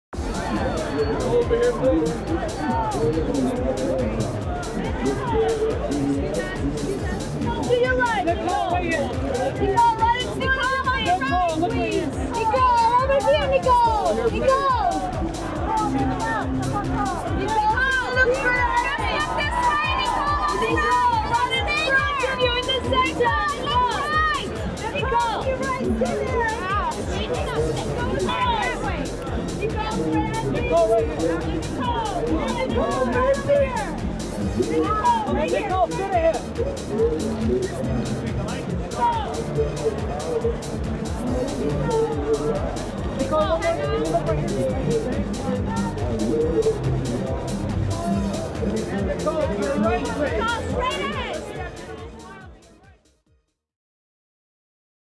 Nicole Dabeau at "The Green Hornet" Premiere
Nicole Dabeau (TV Personality) arrives at "The Green Hornet" Premiere Red Carpet Arrivals at Grauman's Chinese Theatre in Los Angeles,Ca USA January 10, 2011

 © Ricomix Productions /  /